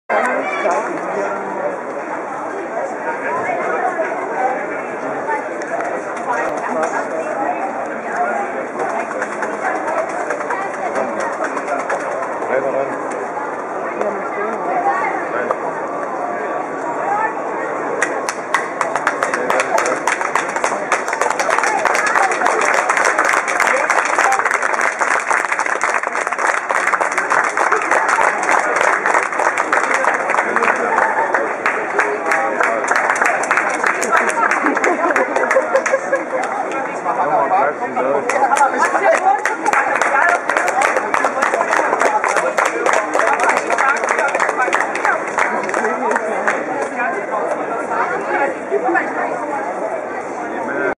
North Korean Team after match USA vs. DPR Korea
Tripod: Sony VCT 60AV
Description:The soccer match surroundings between the USA and North Korea in Dresden, at the women's World Cup 2011.